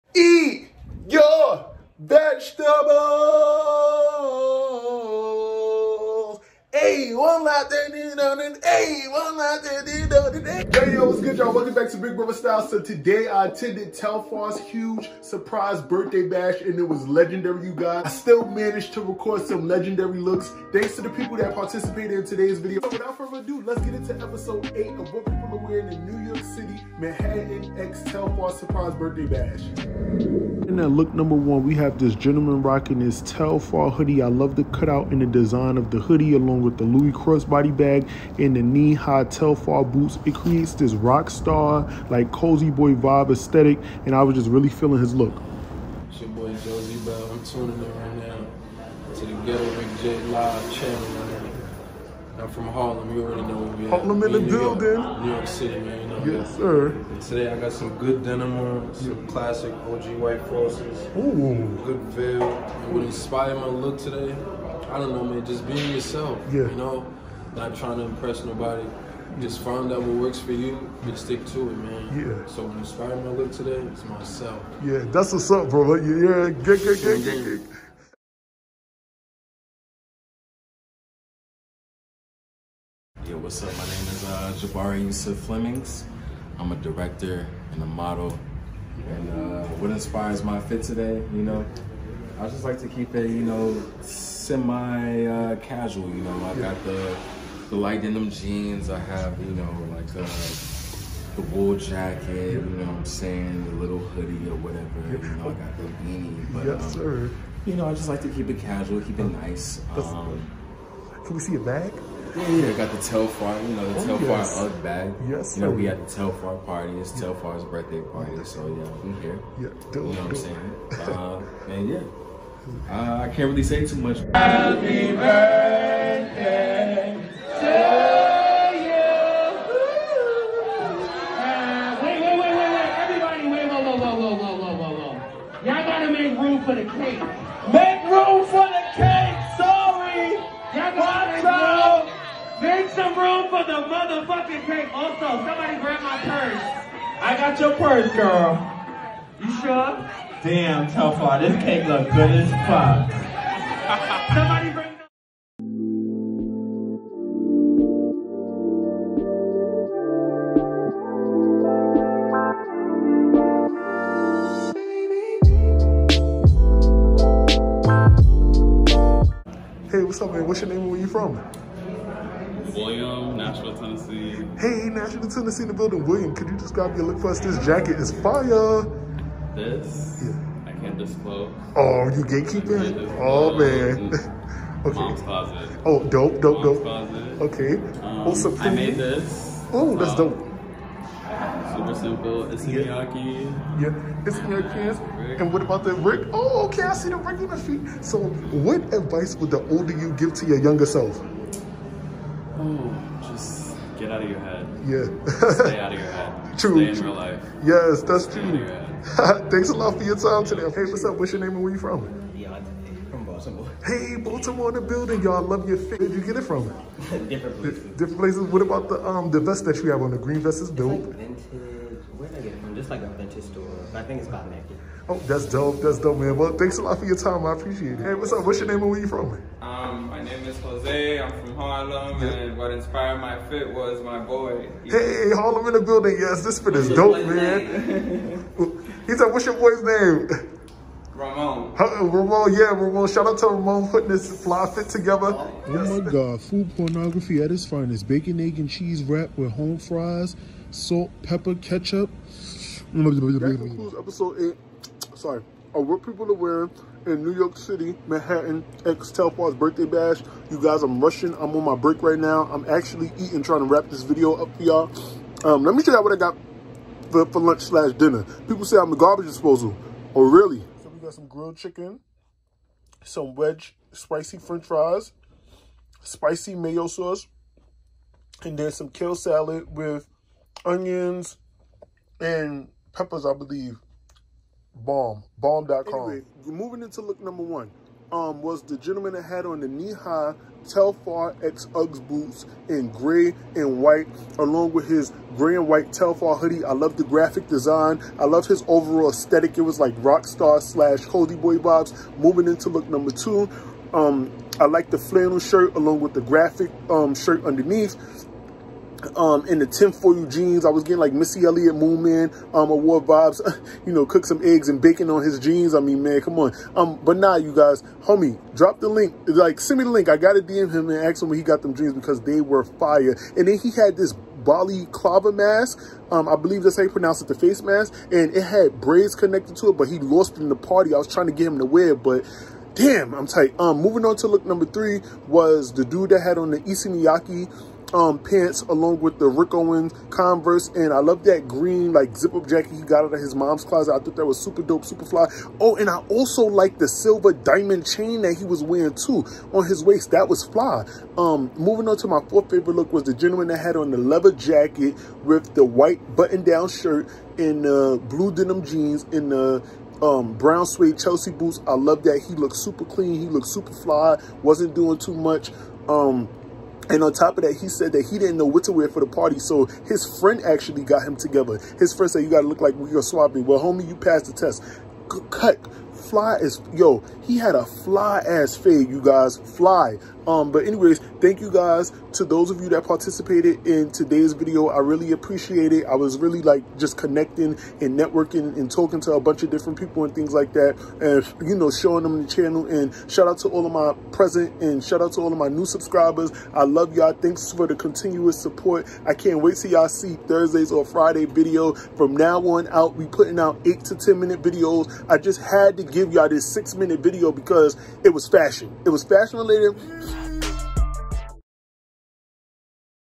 What Are People Wearing In New York City? ft. Manhattan (Telfar Clemens Birthday Bash) (EP.8)
What’s gud you guys Welcome Back To The Ghetto Rick J😎🤙🏾🔥. Hope all is well and everyone is safe and blessed🙏🏾❤️.

Telfarglobal Telfar TelfarClemens this was a dope experience man and even though I wasn’t able to record females outfits because they were a bit revealing I figured I’d still show you guys something 💯👢.

Shoutout to my real ones I love y’all the long way!!!